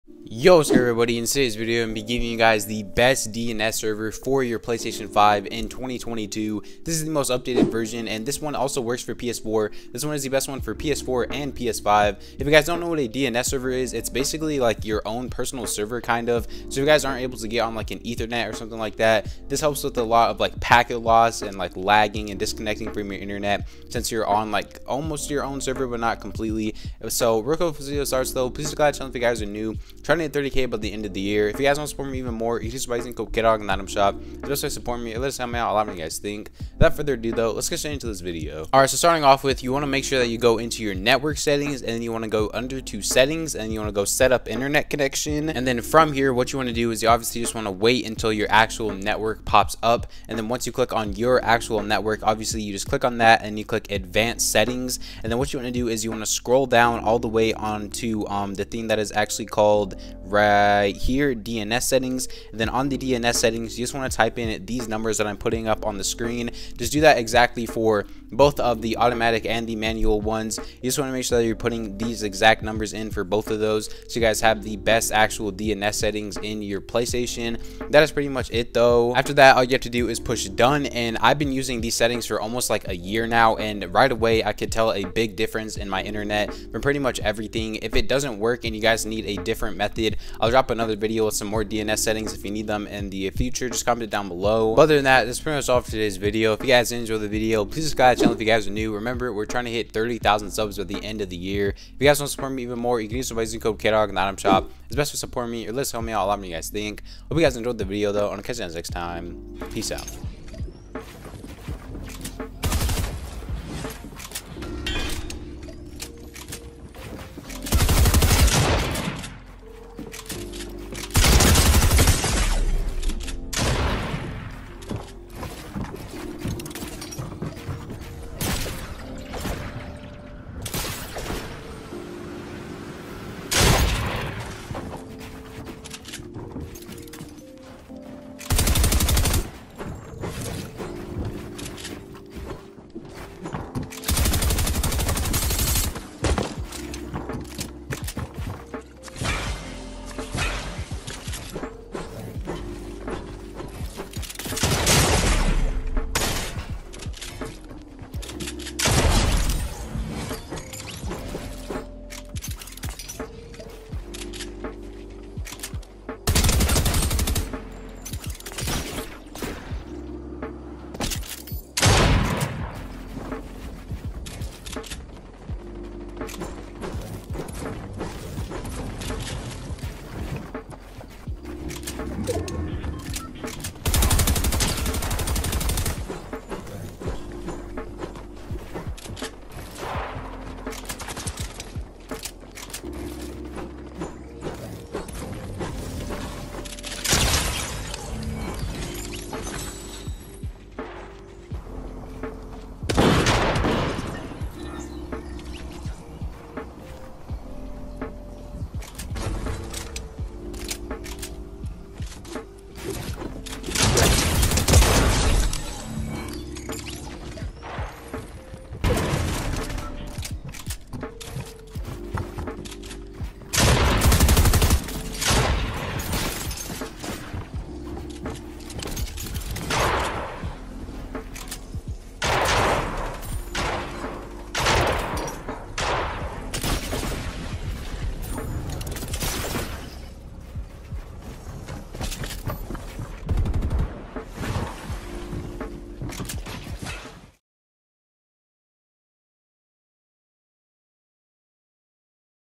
The *BEST* DNS Server For PS4/PS5! How To BOOST Download Speed, REDUCE Ping, and FIX LAG! (2022)
Use Code Kdog in the Fortnite Item Shop